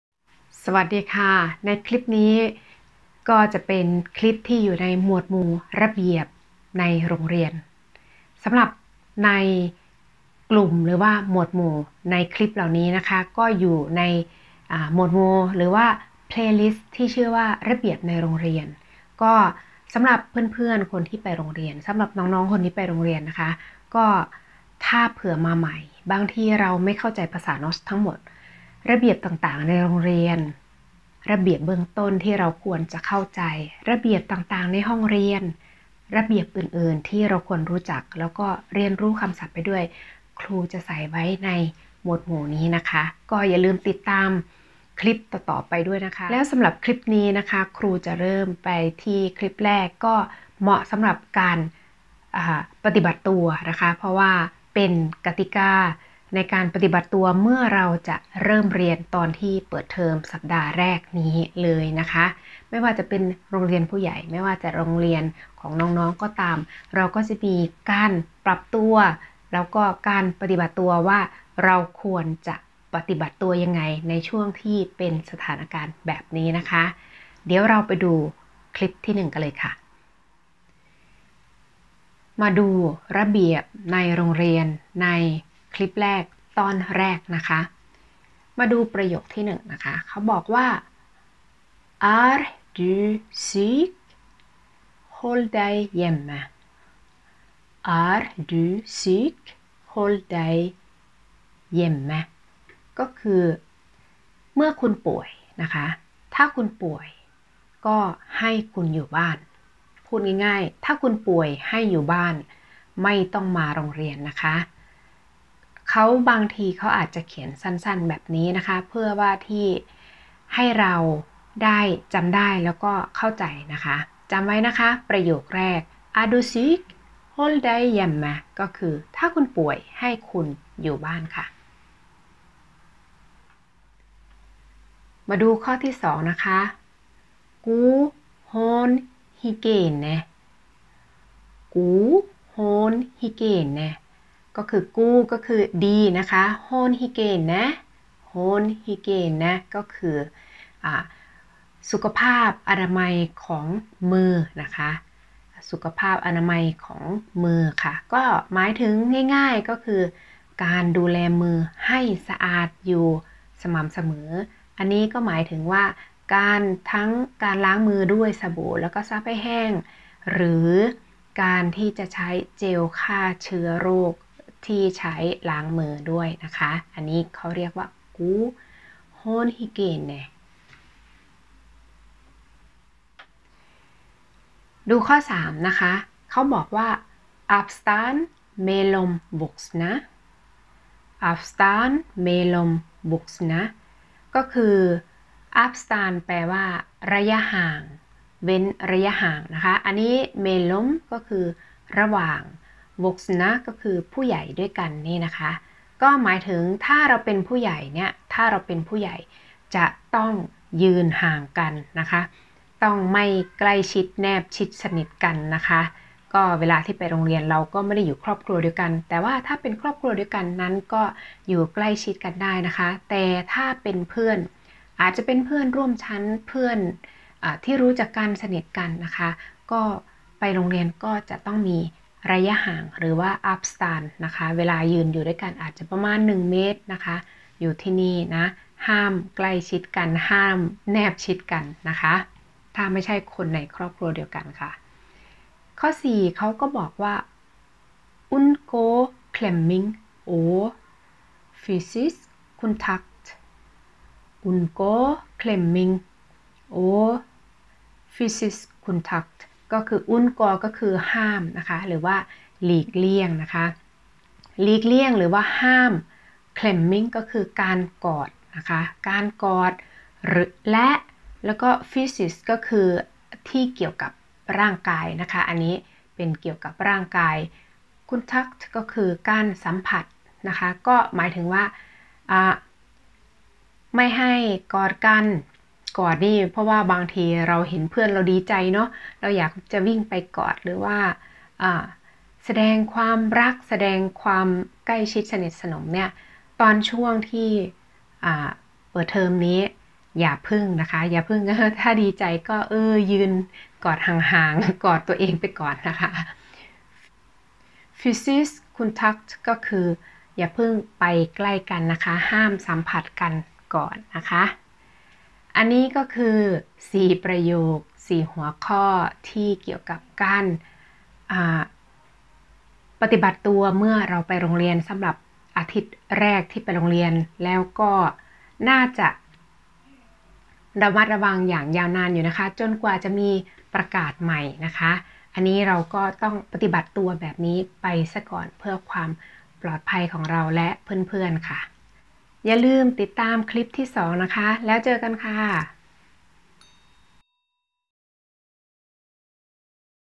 Ep775 ระเบียบ​ใน​โรงเรียน​ที่​นอร์เวย์​ ตอนที่​1
ระเบียบ​ การปฏิบัติ​ตัว เมื่อไปโรงเรียน​ สำหรับ​ผู้​เรียน​ภาษา​นอร์เวย์​พื้นฐาน​ ที่ภาษา​ยังไม่แข็งแรง​ค่ะ